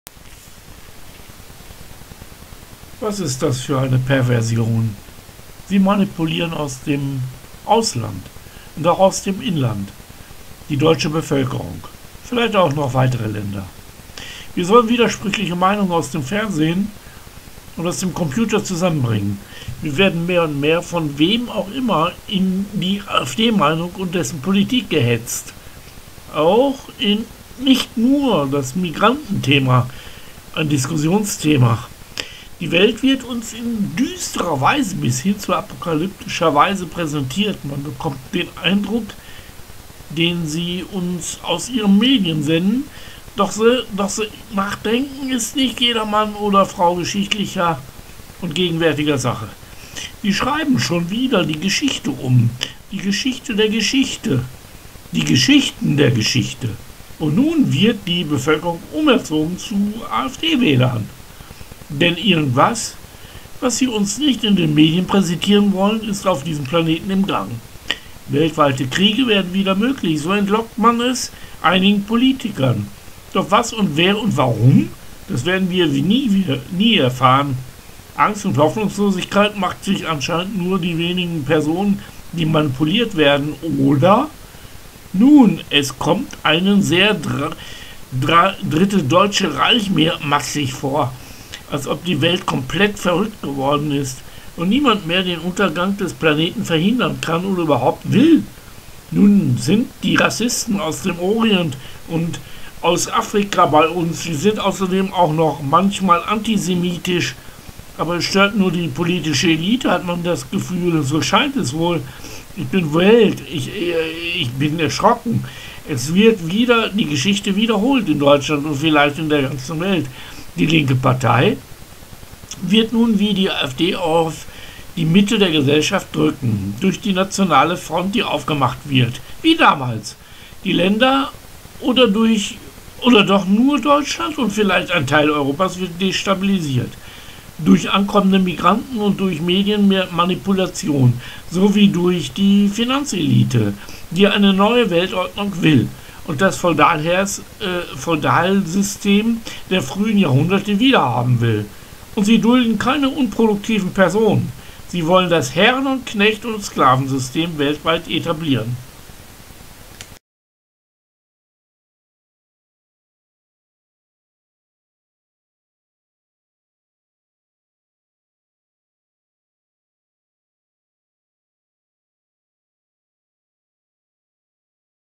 Perversionen in der Welt 820, Texte zum Diskutieren
Foto und Text von Stefan Schrutek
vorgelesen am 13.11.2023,
Das Spiel der Ölbarone und der Wirtschaftsbosse, sowie der Mafien, in der Ganzen Welt, nicht nur die Finanzelite, und die Familienpolitik einiger Länder, die immer mehr Bevölkerung generieren, die dann aber nicht im Lande bleiben können.